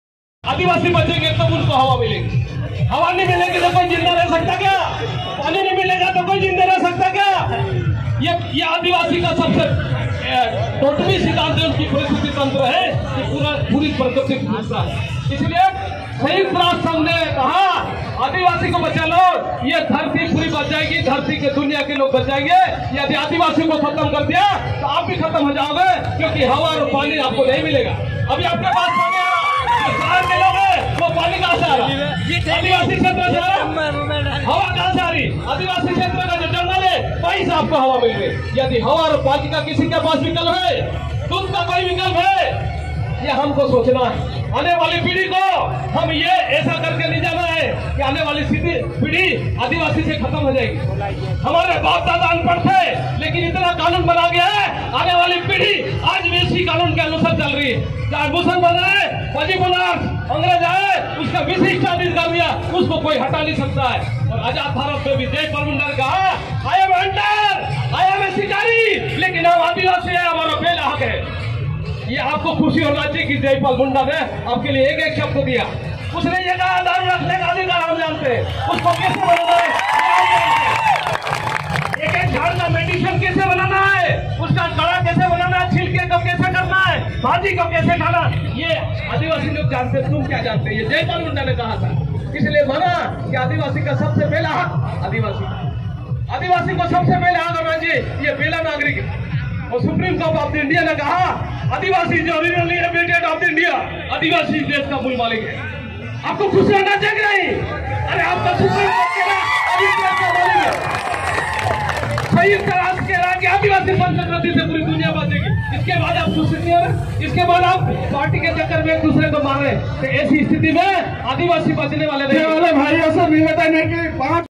9 अगस्त विश्व आदिवासी दिवस कार्यक्रम अलीराजपुर 2022 Alirajpur Madhya Pradesh
Tribal Culture in Alirajpur Madhya Pradesh

जय जोहर जय आदिवासी

अलीराजपुर मध्य प्रदेश में सभी आदिवासी समाज मिलकर मनाया विश्व आदिवासी दिवस 2022

हमारे चैनल पर दिखाई गई वीडियो सिर्फ मनोरंजन के लिए हैं कृपया ध्यान रखे की इस विडियो का गलत उपयोग न करें।